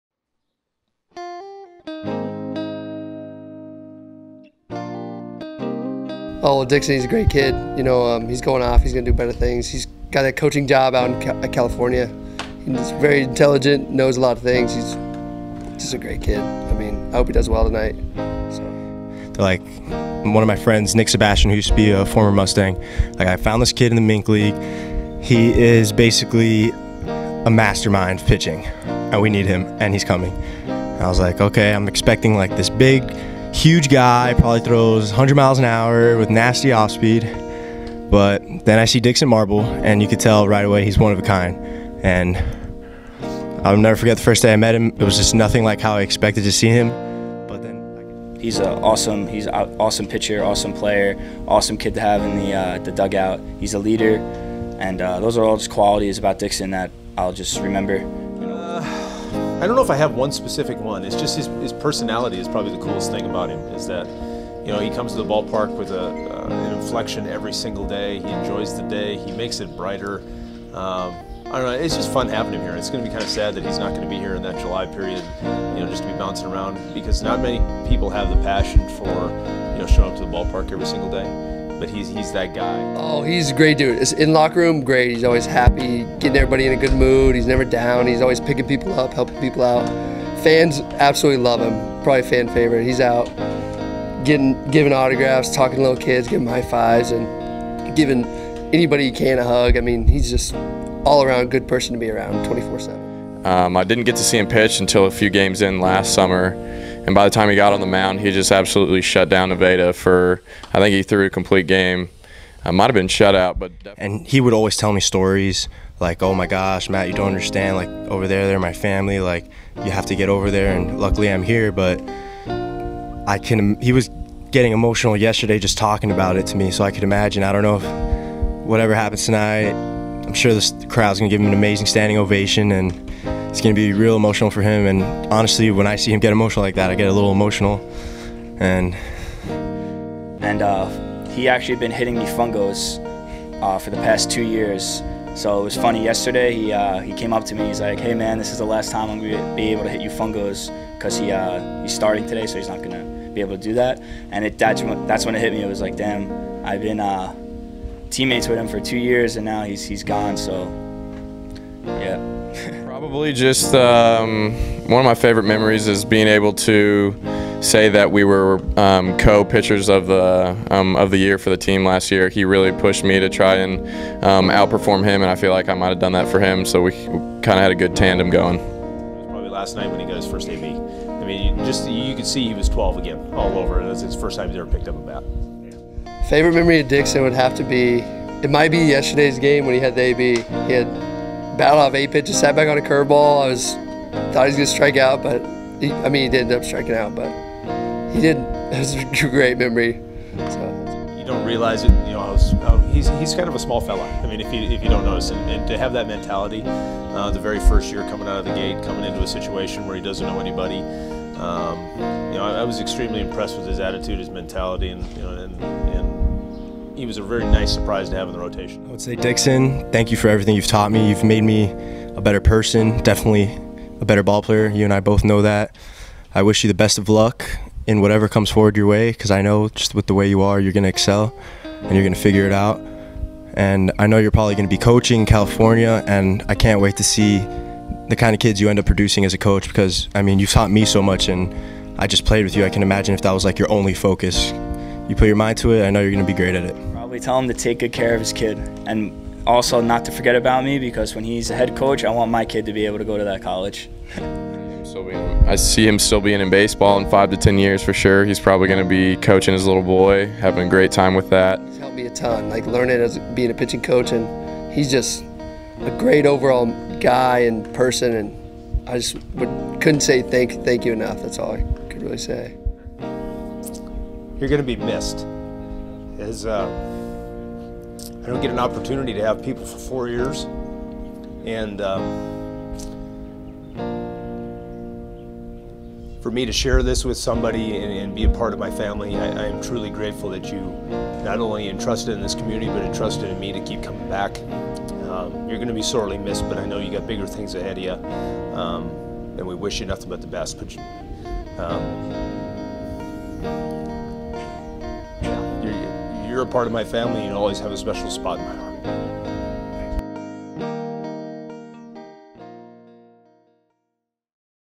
A Farewell to Dixon Marble
Dixon Marble finishes his career at the St. Joseph Mustangs after four years and multiple MINK League Championships. His teammates and coaches took some time to bid him a farewell.